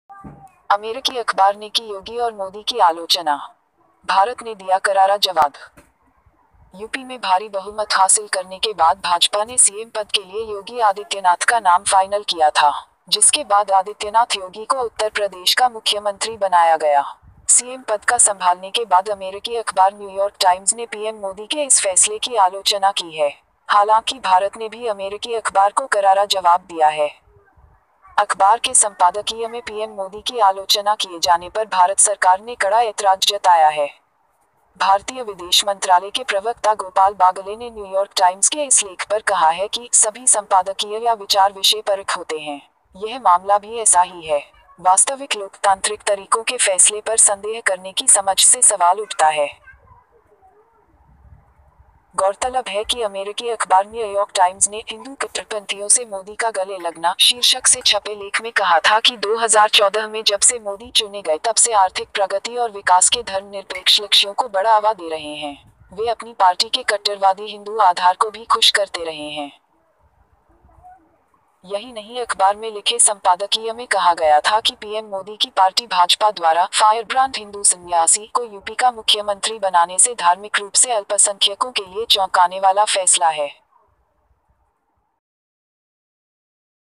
अमेरिकी अखबार ने की योगी और मोदी की आलोचना, भारत ने दिया करारा जवाब
Entry News

        यूपी में भारी बहुमत हासिल करने के बाद भाजपा ने सीएम पद के लिए योगी आदित्यनाथ का नाम फाइनल किया था। जिसके बाद आदित्‍यनाथ योगी को उत्‍तर प्रदेश का मुख्‍यमंत्री बनाया गया। सीएम पद का संभालने के बाद अमेरिकी अखबार न्‍यूयॉर्क टाइम्‍स ने पीएम मोदी के इस फैसले की आलोचना की है। हालांकि भारत ने भी अमेरिकी अखबार को करारा जवाब दिया है।
अखबार के संपादकीय में पीएम मोदी की आलोचना किए जाने पर भारत सरकार ने कड़ा ऐतराज जताया है। भारतीय विदेश मंत्रालय के प्रवक्‍ता गोपाल बागले ने न्यूयॉर्क टाइम्स के इस लेख पर कहा है कि 'सभी संपादकीय या विचार विषयपरक होते हैं। यह मामला भी ऐसा ही है। वास्तविक लोकतांत्रिक तरीकों के फैसले पर संदेह करने की समझ से सवाल उठता है।'
गौरतलब है कि अमेरिकी अखबार न्ययॉर्क टाइम्स ने 'हिंदू कट्टरपंथियों से मोदी का गले लगना' शीर्षक से छपे लेख में कहा था कि 2014 में जब से मोदी चुने गए तब से आर्थिक प्रगति और विकास के धर्मनिरपेक्ष लक्ष्यों को बढ़ावा दे रहे हैं। वे अपनी पार्टी के कट्टरवादी हिंदू आधार को भी खुश करते रहे हैं।
यही नहीं अखबार में लिखे संपादकीय में कहा गया था कि पीएम मोदी की पार्टी भाजपा द्वारा 'फायरब्रांड हिंदू संन्‍यासी' को यूपी का मुख्‍यमंत्री बनाने से धार्मिक रूप से अल्‍पसंख्‍यकों के लिए चौंकाने वाला फैसला है।